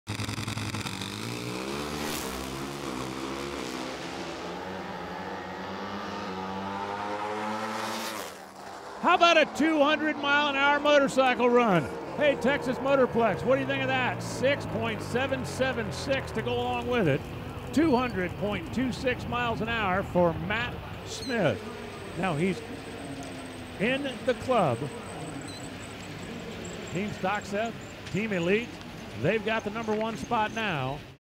Matt Smith's 200-mph blast rockets him to the top qualifier position
Reigning world champ Matt Smith flexed the muscles of his Denso-backed EBR, cracking off the first 200-mph Pro Stock Motorcycle pass at Texas Motorplex en route to grabbing the No. 1 qualifying spot.